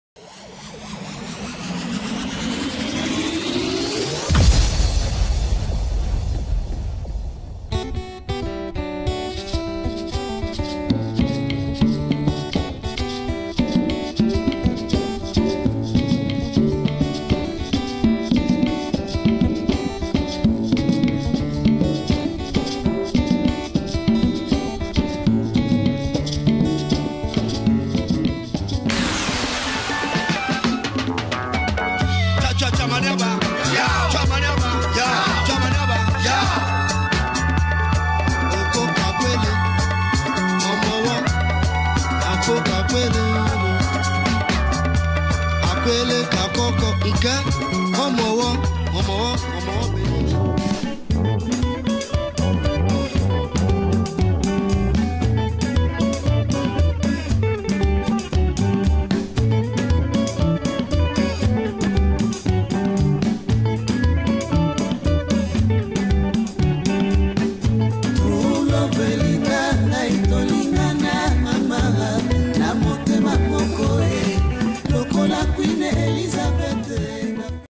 26th August 2018, Ritzy
Upstairs at the Ritzy in Brixton, we have the most amazing musicians from Central Africa and South Africa. Come, have an African good times!